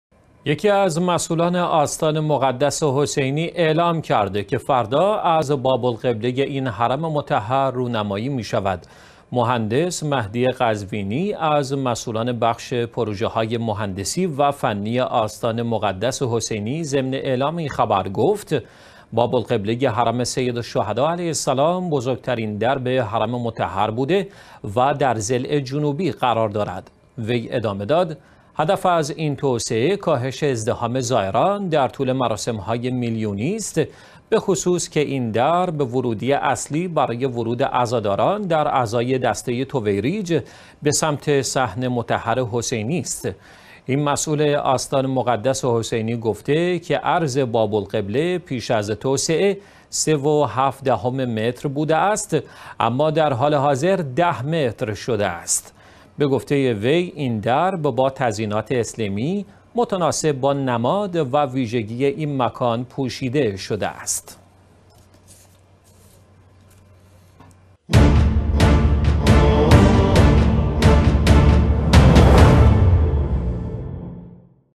افتتاح بزرگ‌ترین باب حرم امام حسین علیه السلام
یکی از مسئولان آستان مقدس حسینی اعلام کرده که فردا از باب القبله این حرم مطهر رو نمایی می شود.
مهندس "مهدی قزوینی" از مسئولان بخش پروژه های مهندسی و فنی آستان مقدس حسینی ضمن اعلام این خبر گفت: باب القبله حرم سید الشهدا علیه السلام، بزرگ‌ترین در حرم مطهر بوده و در ضلع جنوبی قرار دارد.
وی ادامه داد: هدف از این توسعه، کاهش ازدحام زائران در طول مراسم های میلیونی است، به خصوص که این در، ورودی اصلی برای ورود عزاداران در عزای دسته طویریج به سمت صحن مطهر حسینی است.
این مسئول آستان مقدس حسینی گفته که عرض باب القبله پیش از توسعه، 3.7 متر بوده است، اما درحال حاضر 10 متر شده است.
به گفته وی این در با تزئینات اسلیمی متناسب با نماد و ویژگی این مکان پوشیده شده است.